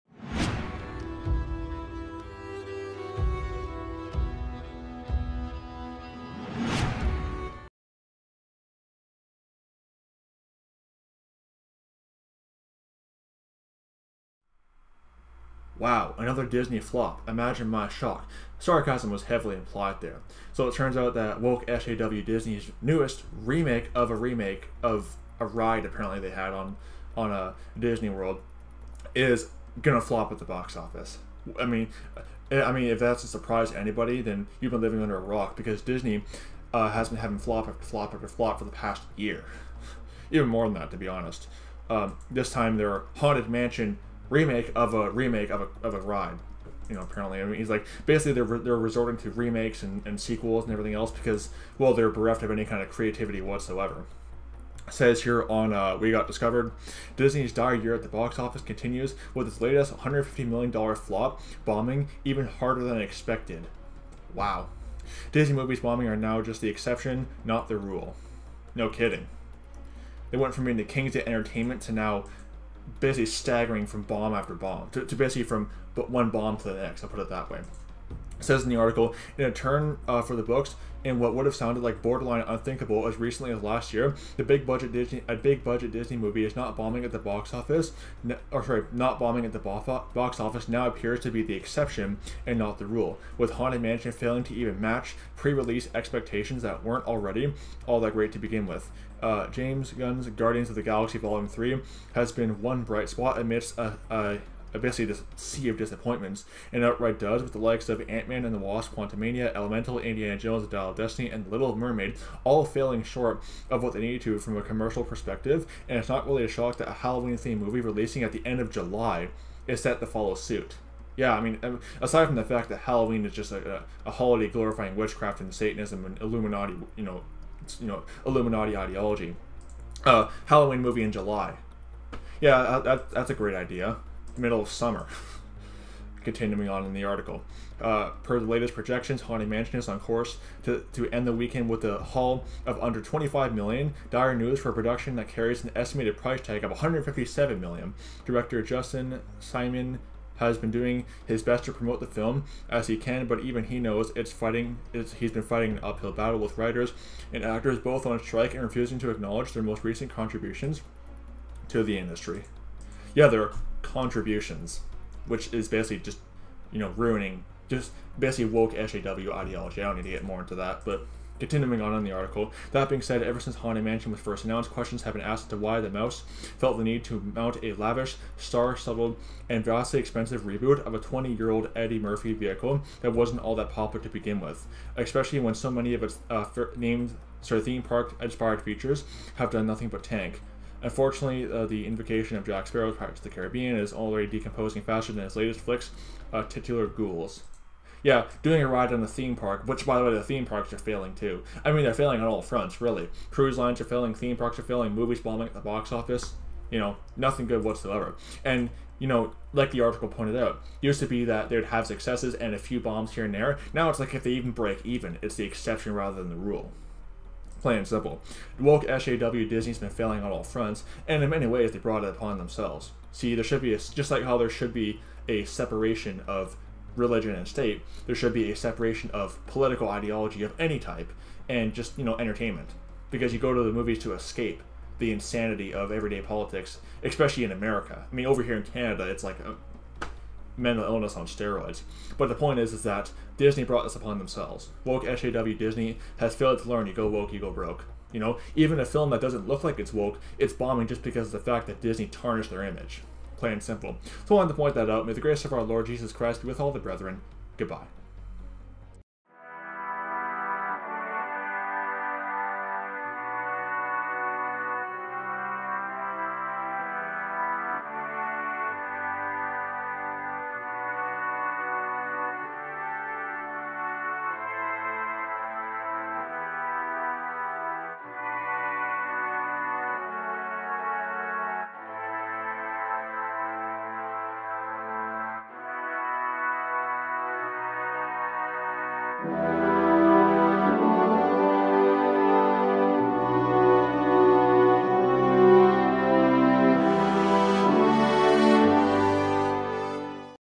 Disney's Haunted Mansion Remake Set To Flop | Woke-SW Disney FAIL
Woke-SJW Disney’s haunted mansion remake is set to join Disney’s long list of fails!